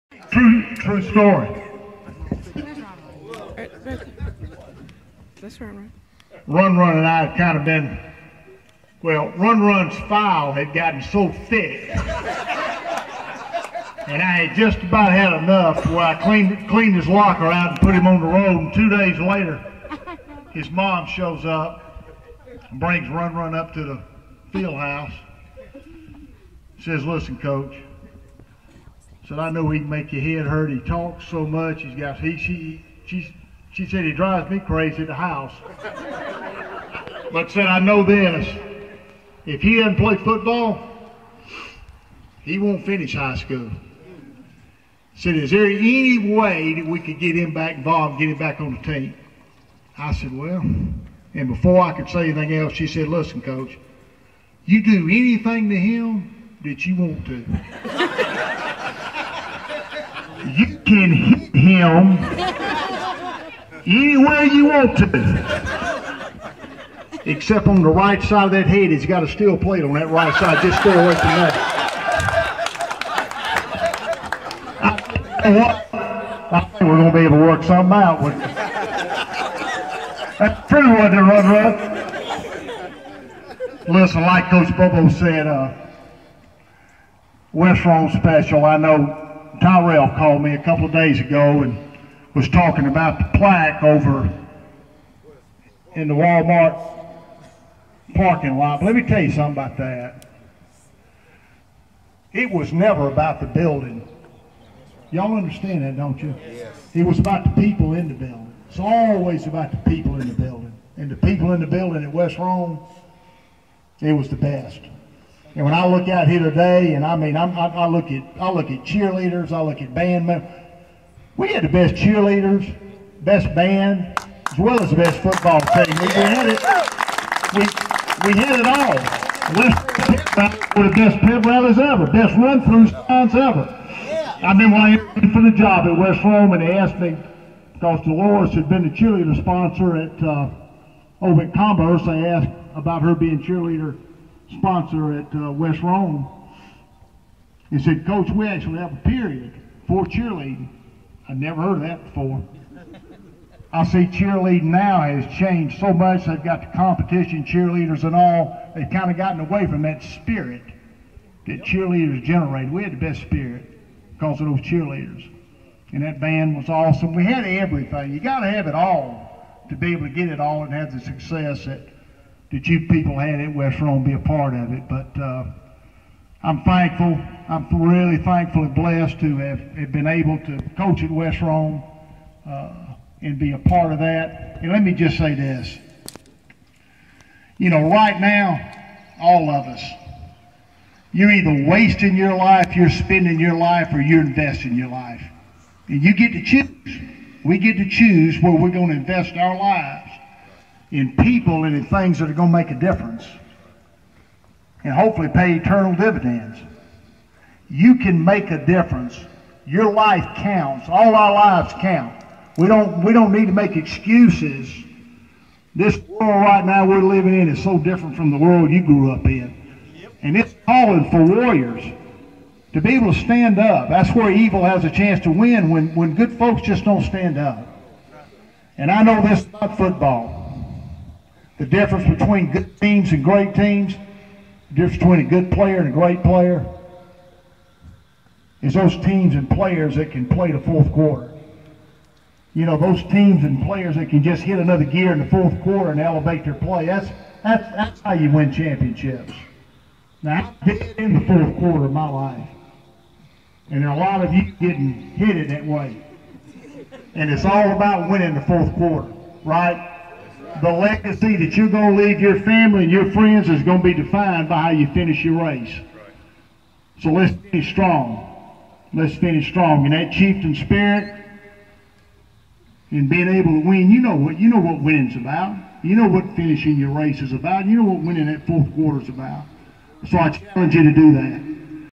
Mike Hodges, West Rome High School Chieftains Head Coach
Opening Remarks during Pep Talk at 2016 Chieftain Rumble, Rome GA